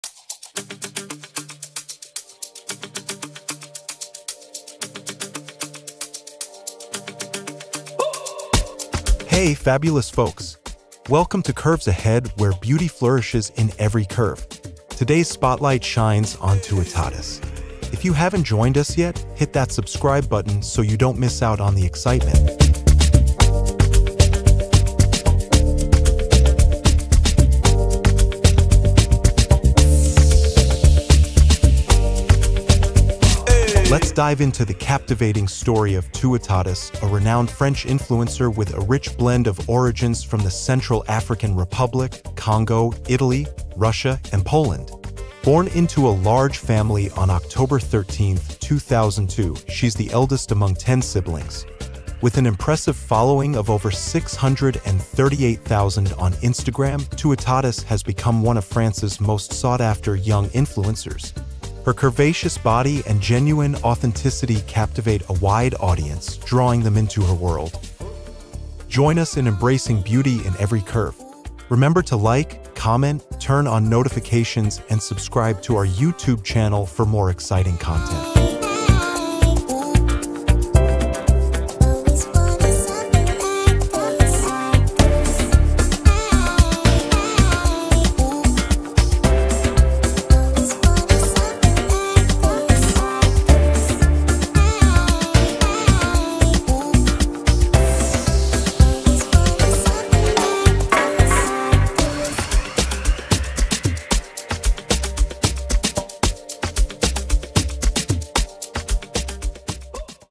Tootatis : Curvy Plus Size Instagram Model | Fashion | Bio | Wiki | Facts
Join us as we uncover Tootatis' bio, wiki, and fascinating facts that make her a true icon in the fashion industry. Whether you're a fashion enthusiast or someone looking for a boost of confidence, this video is for you!